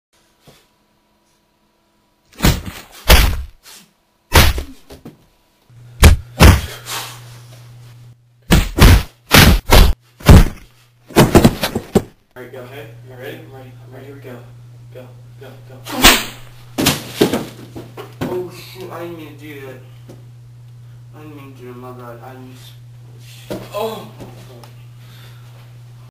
Testing out 2 person Video editing
I managed to get two of me in a video im just testing out what I can do with this lil feature tell me what u think!!